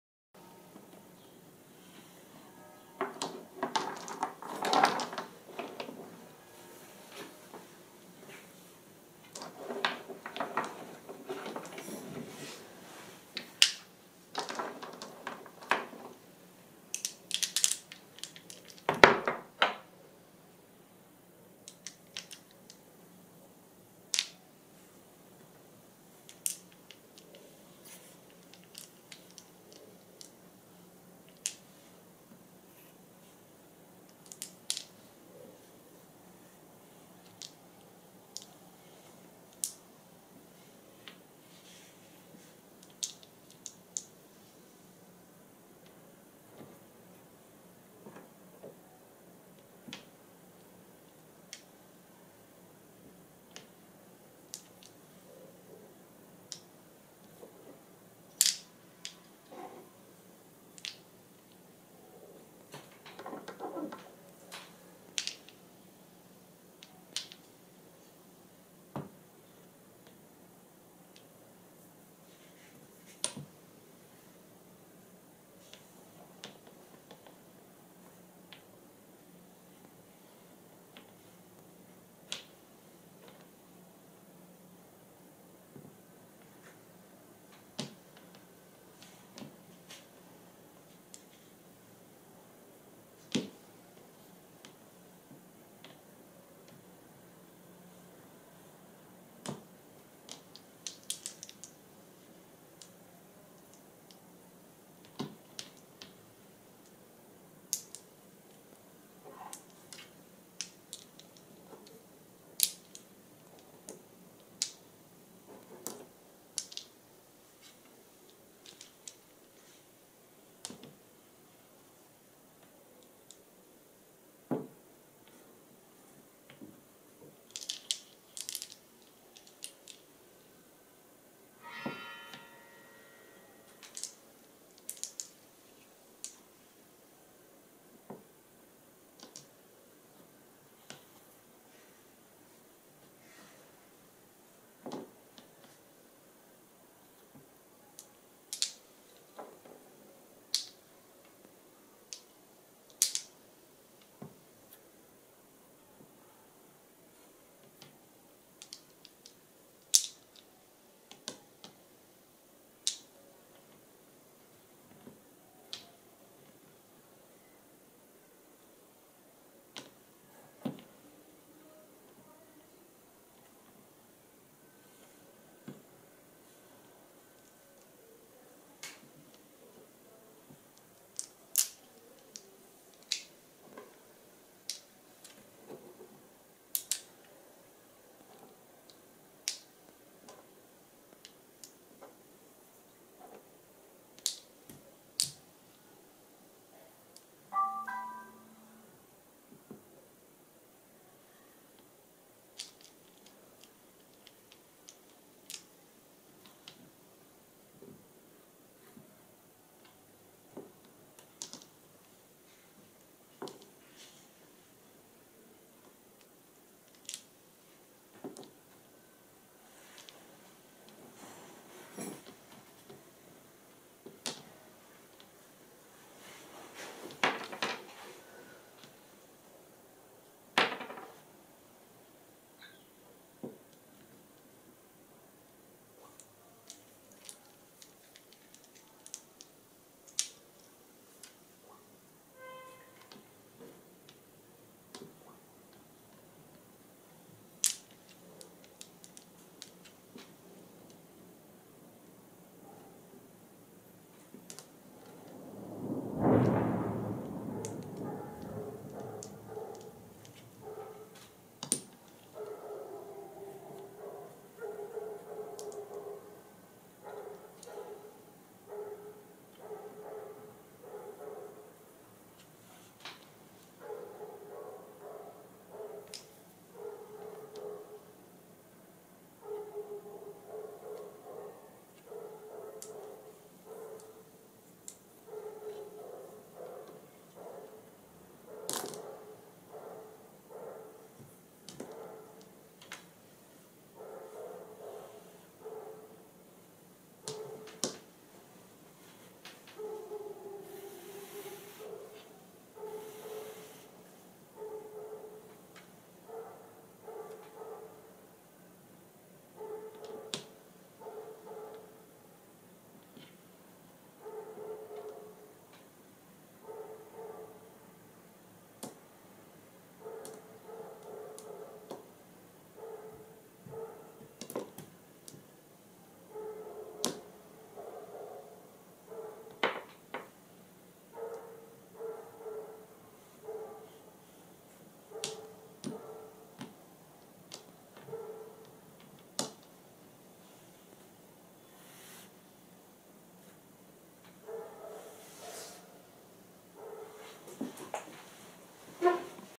ASMR - White stones in circle
Fitting white stones into a circle made of cork. Stone sounds.